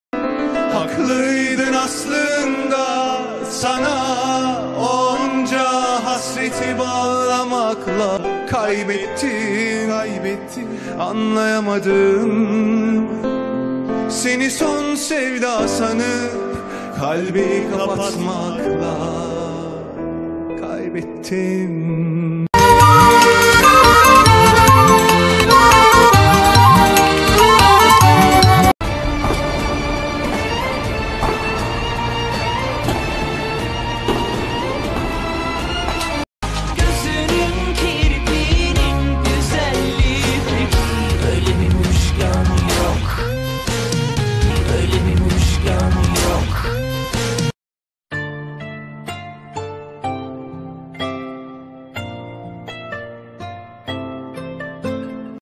Halil Ibrahim Ceyhan✞︎Yaman Kirimli ✔︎klip
Arkadaslar halil ibrahim ve yamanin kliplerini birlwsdirdim sizce nasil olmus??